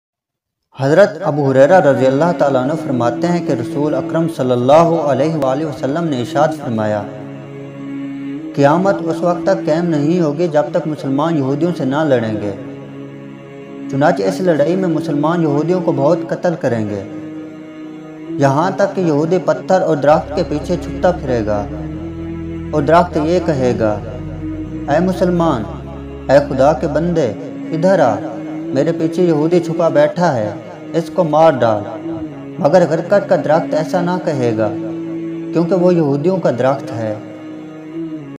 Qayamat kab Aayegi ||| Qayamat ki Nishani ||| Yahodin ka Darakht konsa Hae
Is video mein qayamat ke bare bataya gaya hae keh qayamat kab aai gi aur qayamat ane se phly ki nishai ke bare mein bataya gaya hae